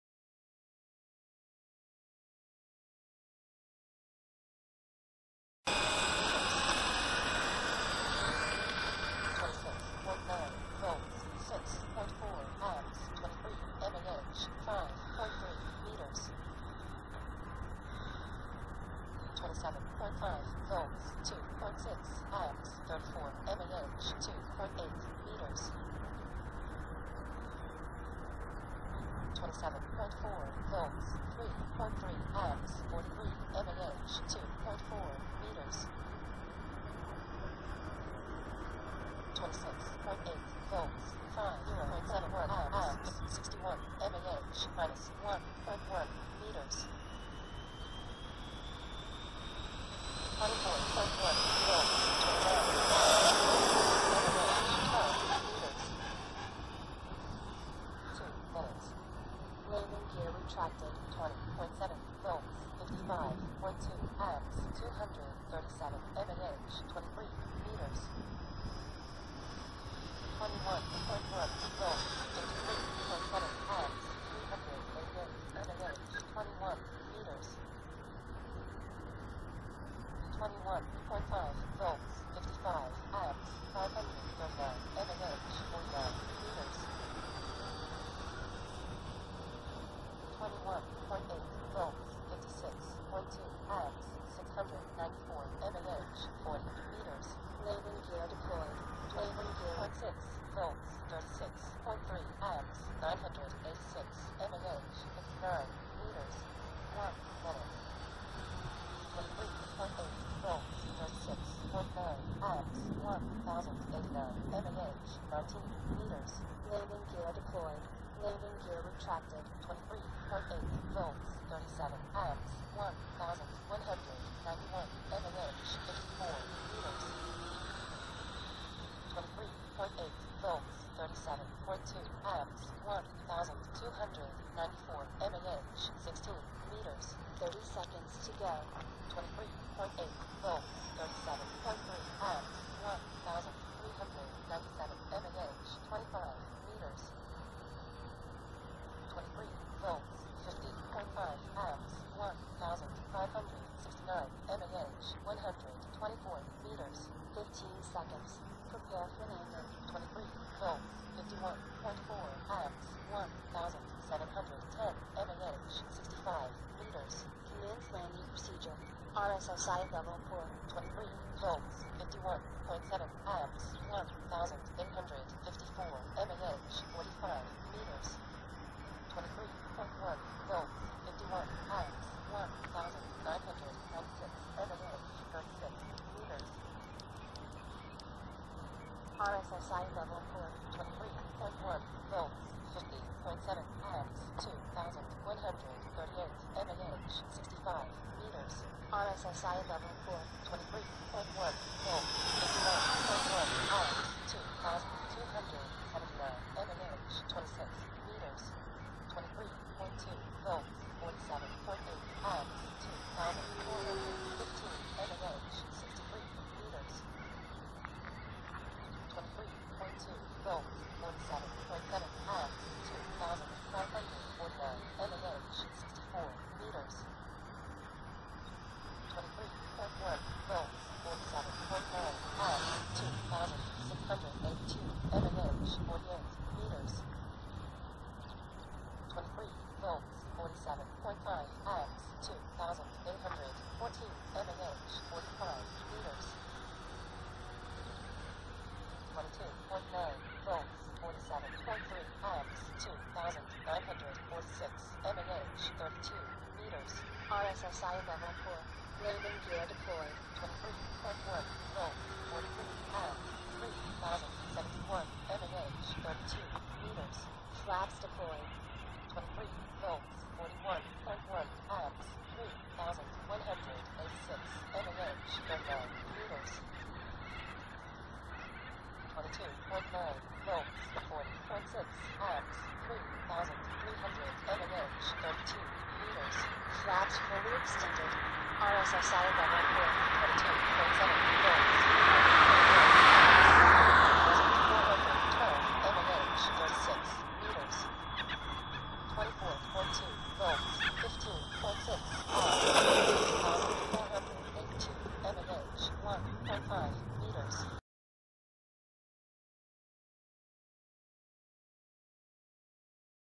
F-4 Phantom - FlyFly 90mm - 7S HK Fandrive SK3968-1500kv CS12
The F-4 Phantom returns, after its "Fly off onto the distance and crash" incident.
It flies the same as ever, but I won't be going further rearwards CofG than the stock 180mm, which was totally fine anyway!
I have added a Mix for Tailerons, just in case it needs 'extra' control authority ever again!

It is not 'finished' fully, so it needs some filling and painting still, but I am also going to add a Wing Spar plus I will most likely move the main gear a bit more forwards and closer to the CofG, so that it can Rotate easier and allow smoother 'non-leaping' take-offs.